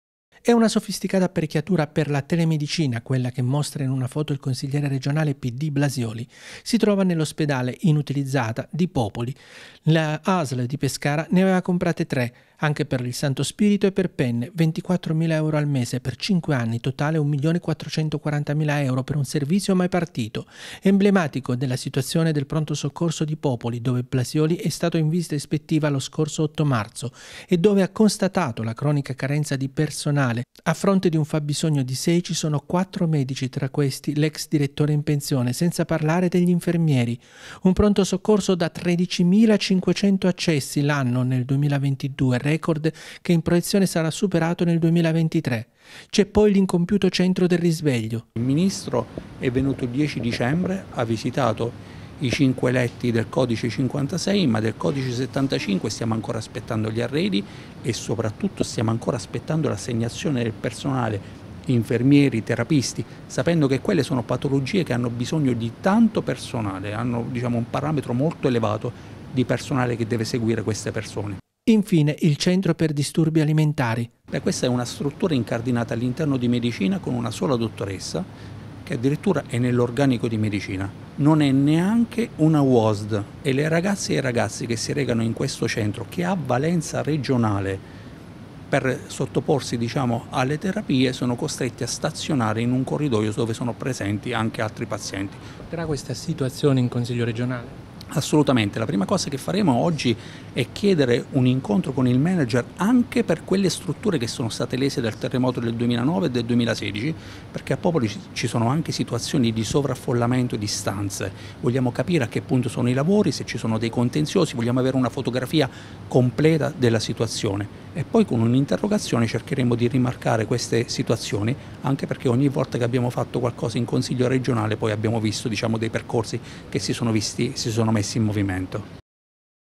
Sanità, Blasioli (PD): “Criticità e sprechi all’ospedale di Popoli”
| SUPER J |

Contatti Redazione:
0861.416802